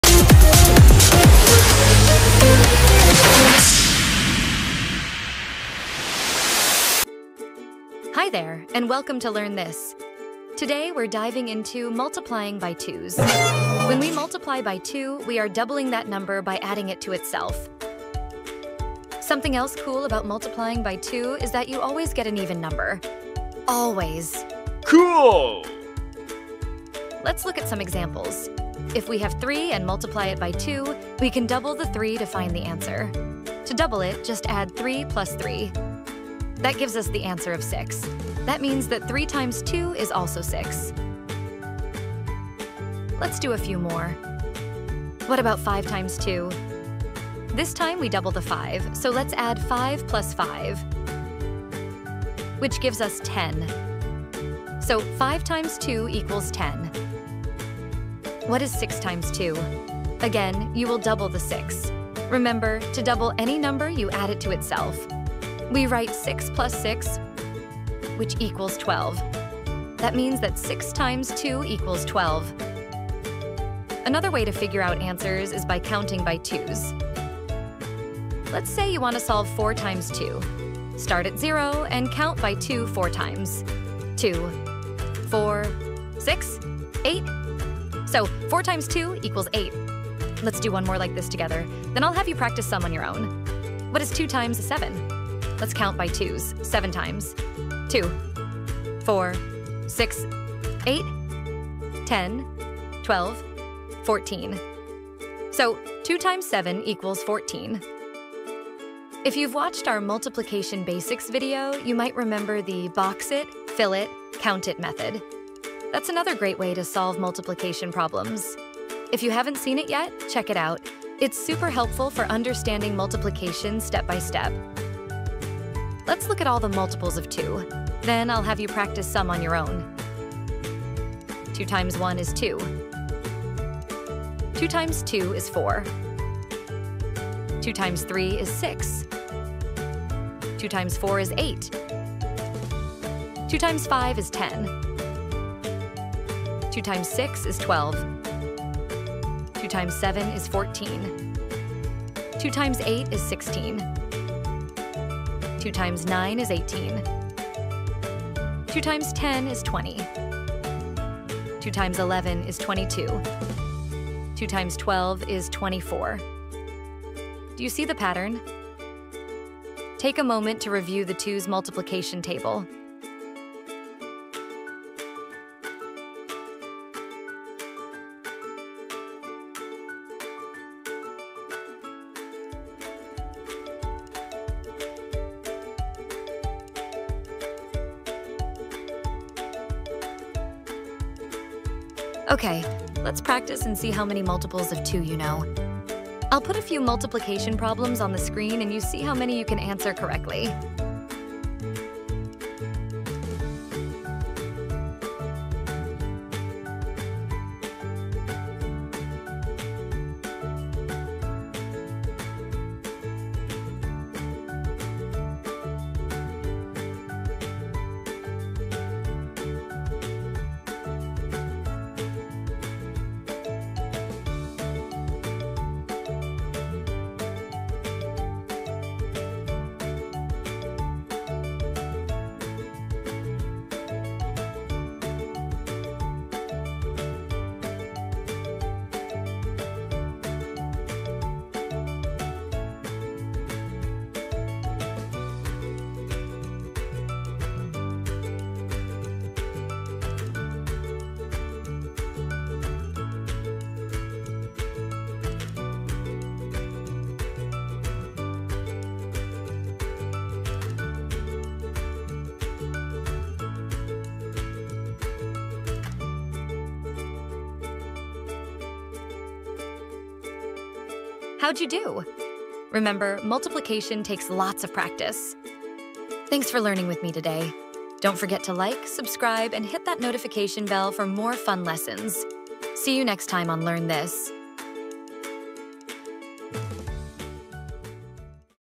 Master Multiplying by 2: Easy Methods & Fun Practice!
Learn how to multiply by 2 in this fun and simple video! We’ll show you how doubling a number makes multiplication easy, and we’ll practice counting by 2s to get the answers quickly. Whether you’re just starting with multiplication or looking for a quick refresher, this video has everything you need to succeed. Plus, if you want to learn about the Box It, Fill It, Count It method, check out our Basics of Multiplication video linked below! Perfect for kids, parents, and teachers.